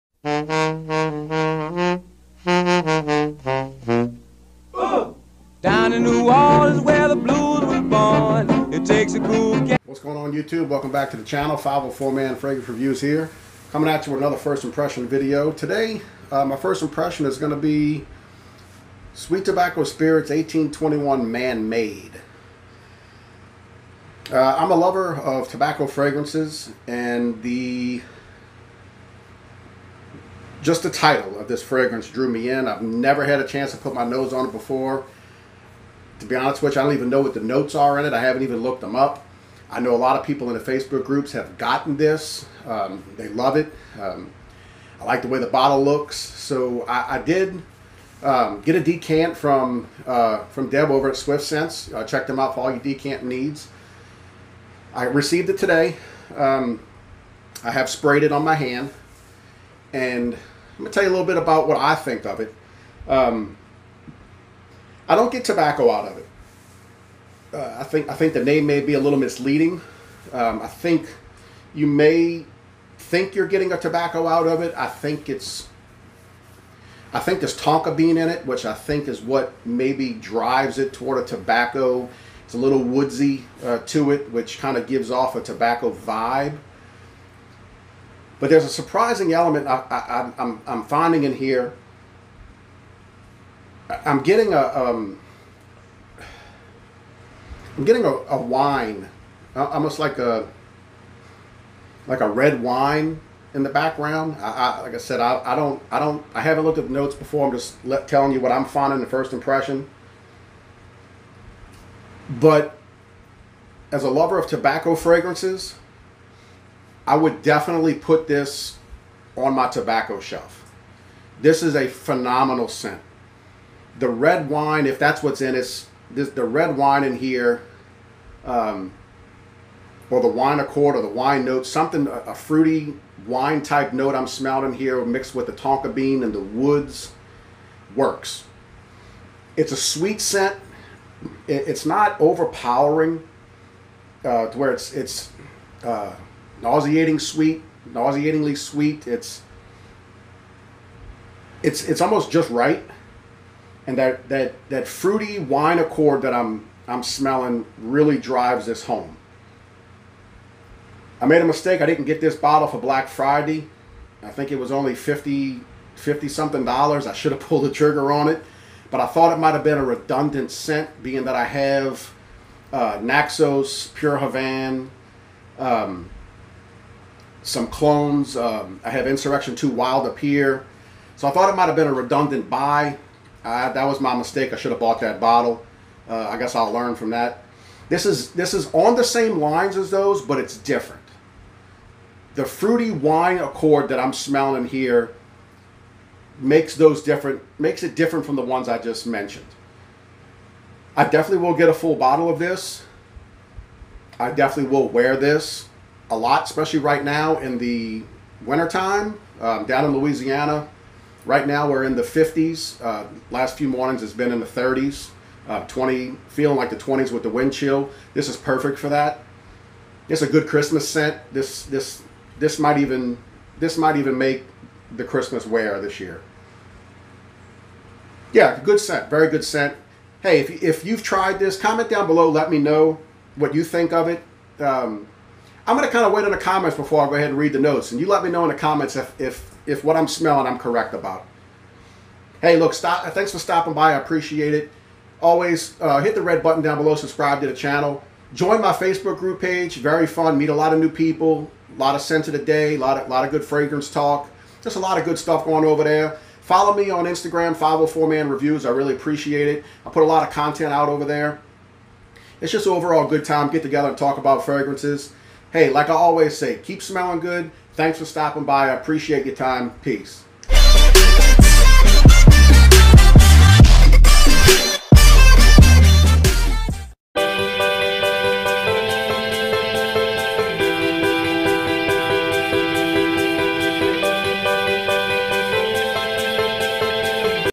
1821manmade First Impression 18.21 Man Made Sweet Tobacco Spirits Fragrance Review
Welcome back to the channel. Today I give you my first impression of 18.21 Man Made Sweet Tobacco Spirits.